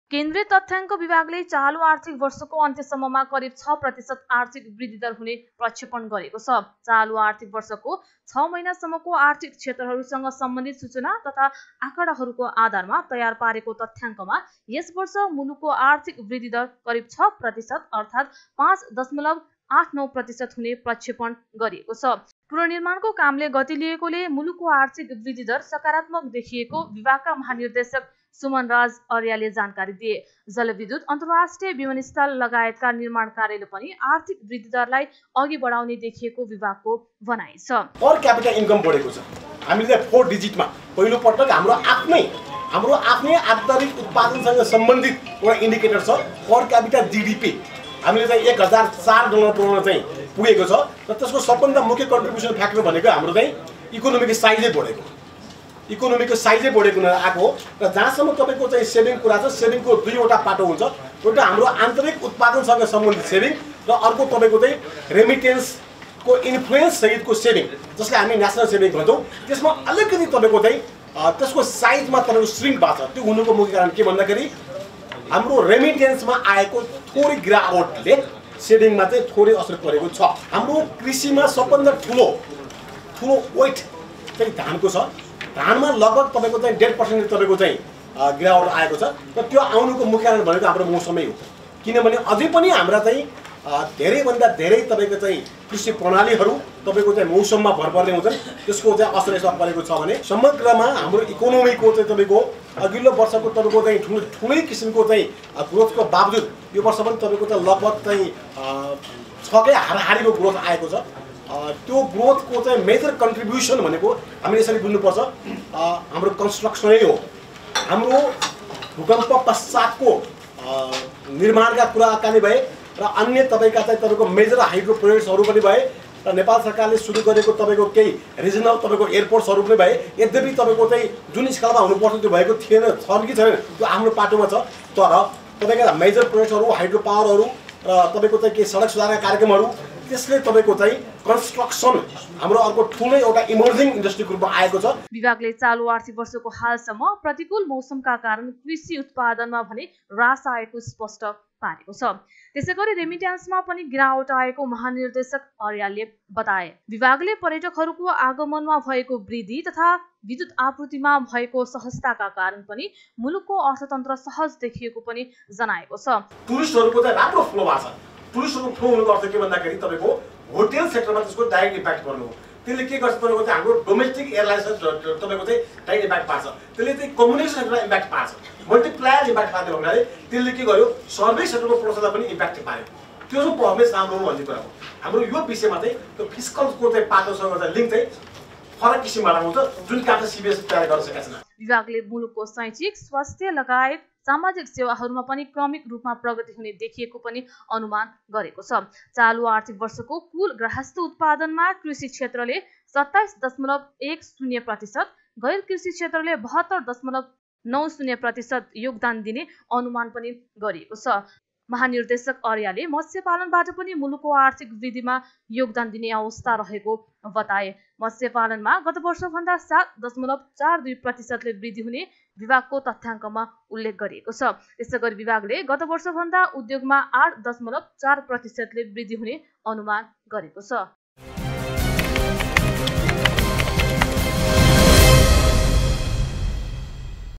प्रतिव्यक्ति आय १ हजार डलर नाघ्यो
तथ्याङ्क विभागका अनुसार प्रतिव्यक्ति आय १ हजार ४ अमेरिकी डलर पुगेको छ।
यसको अर्थ औसतमा एक व्यक्तिले वार्षिक करिब १ लाख ६ हजार रुपैयाँ (डलरसँगको बिनिमयदर १०५.५ रुपैयाँ अनुसार) आम्दानी गर्दछ।